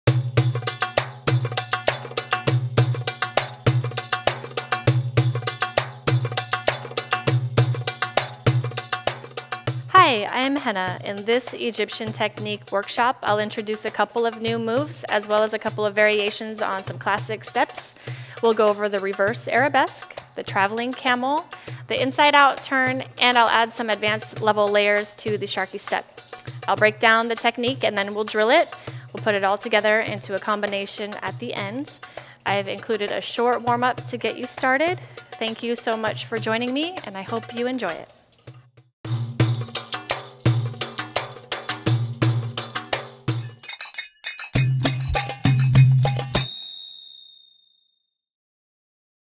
Classic Egyptian Belly Dance Class 3 Preview with Henna
This Egyptian technique workshop introduces more movements from Egyptian style belly dance: the camel, the reverse arabesque, and the inside out turn. We revisit the sharki step to add some advanced layers. A few variations will accompany each break-down and we will cycle through some drills to get them into your muscle memory. Finally, the movements will also be danced together in a short combination. This workshop includes a short warm up to get you ready to dance.